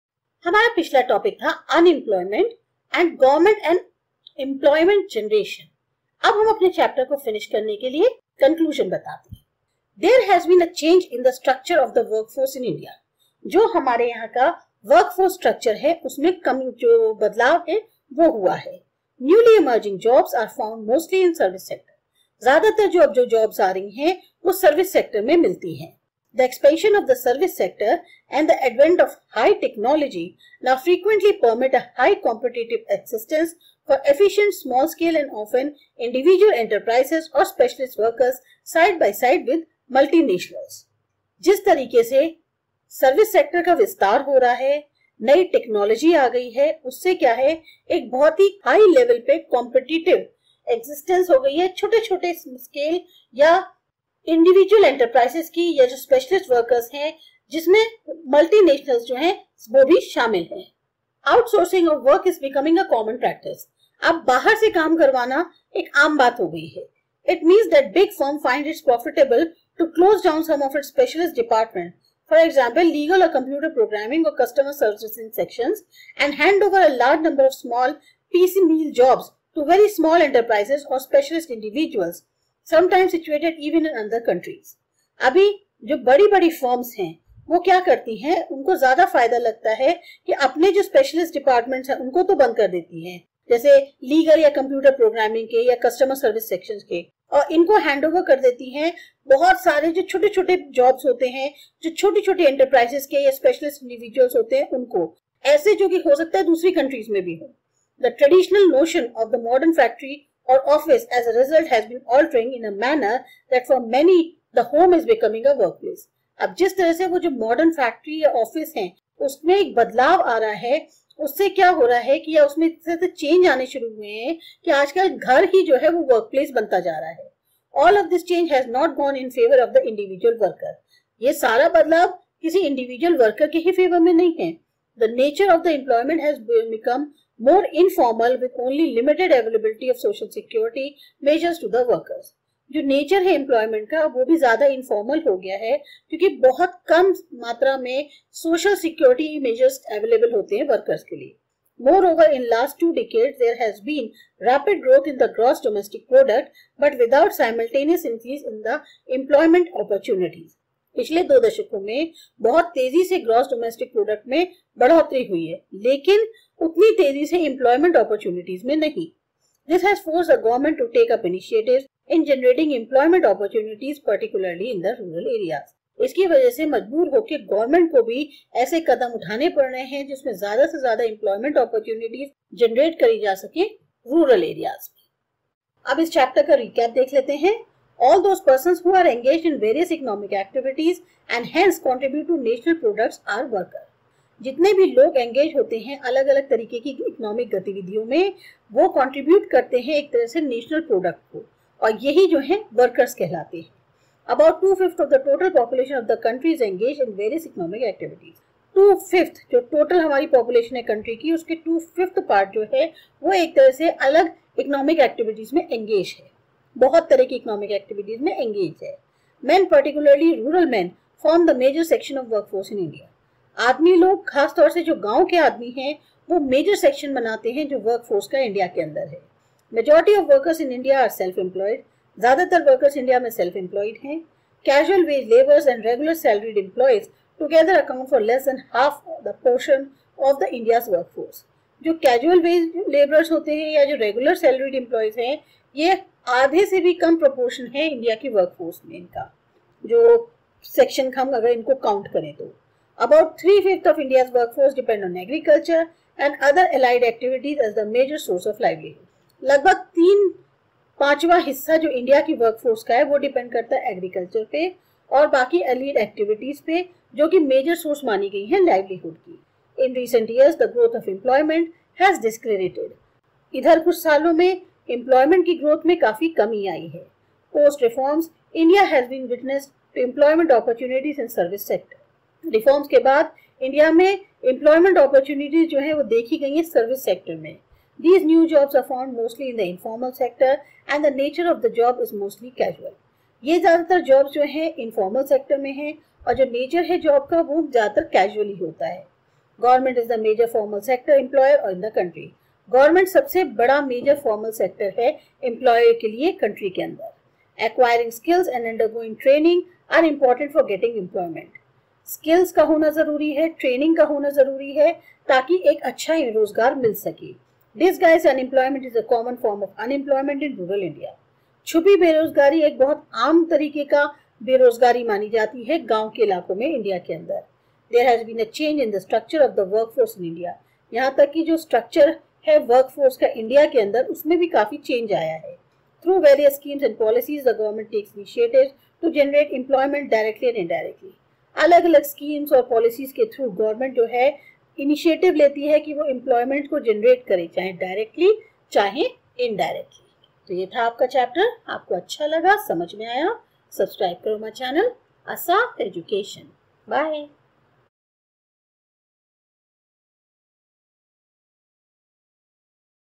Ch 7.6 Employment: Growth, Information & other Issues (Economics, Grade 11, CBSE) Easy Exp in Hindi
Subject - Economics.
Book - Indian Economic Development.
Chapter 7 - Employment: Growth, Information and other Issues.

ASAP-A05